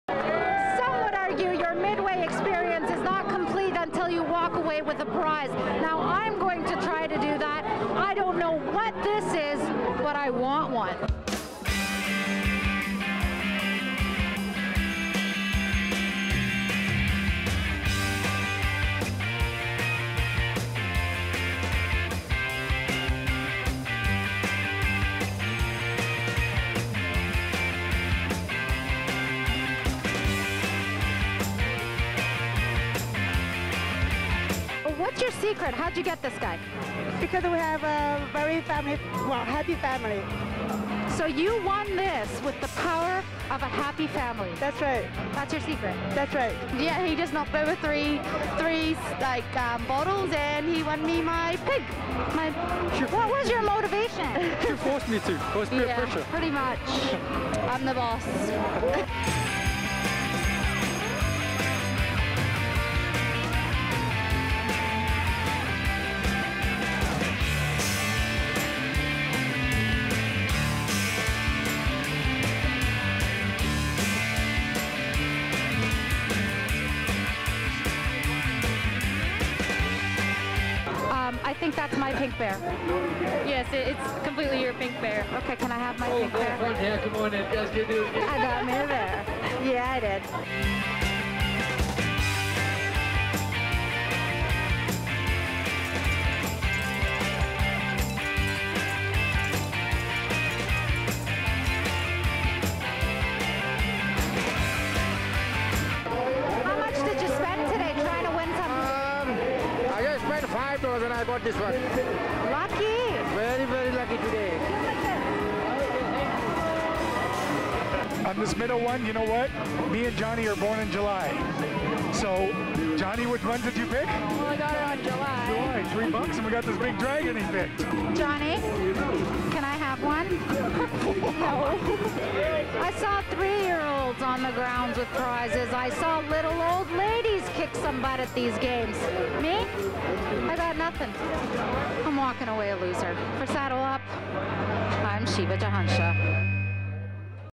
Midway Prizes!
Playing games on the midway is a big part of the Stampede festivities - and even more exciting when you actually win a prize!  Shiva is going to attempt to do just that!